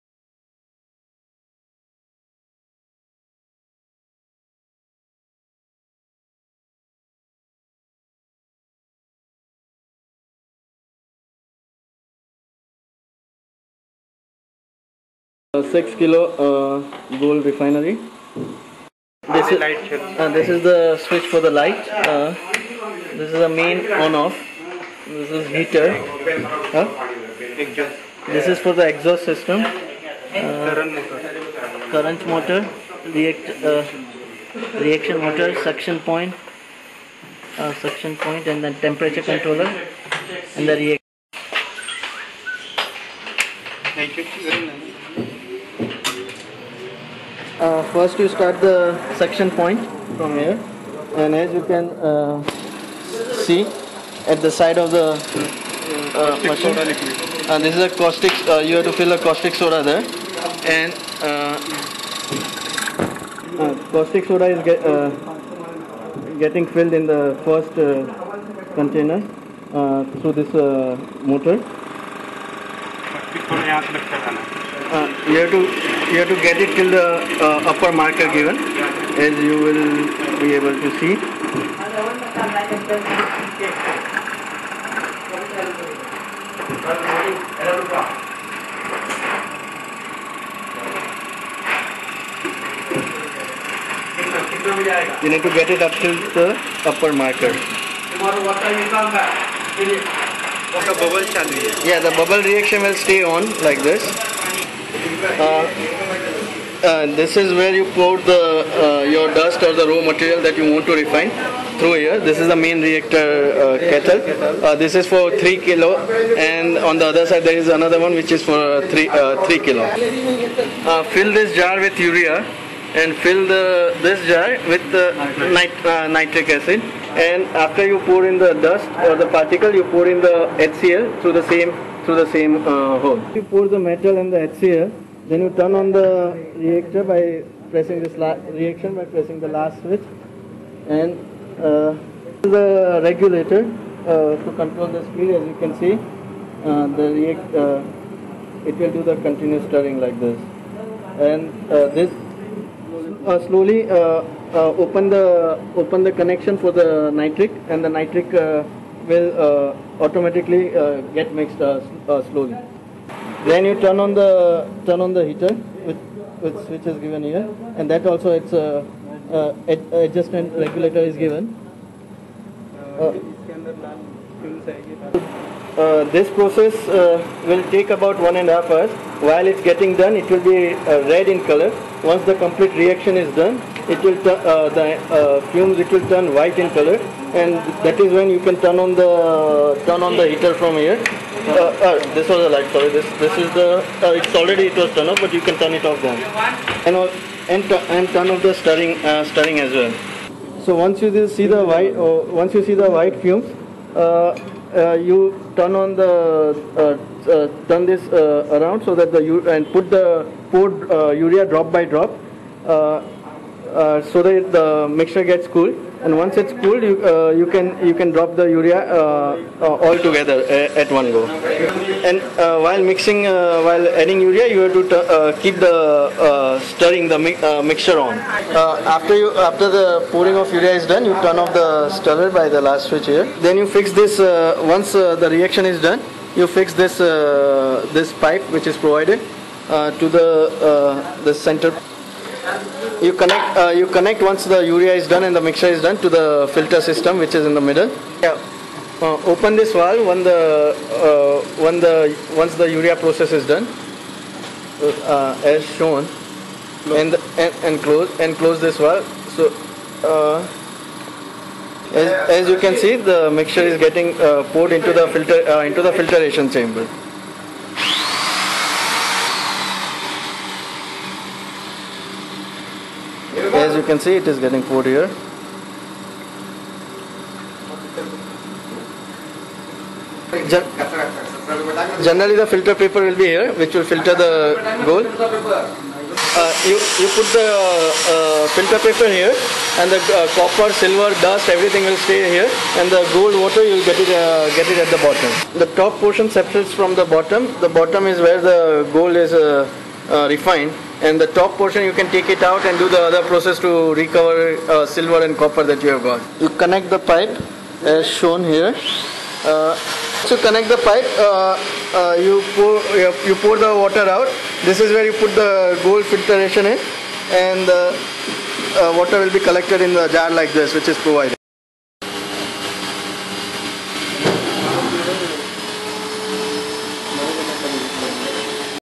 MASTER 6kg capacity Gold Refinery Machine by Machine Tool Traders
This is 6kg capacity Gold Refinery. We can supply you gold refinery from 1kg up to 6kg at a time and silver refinery from 10kg up to 40kgs at a time. Kindly ask for details.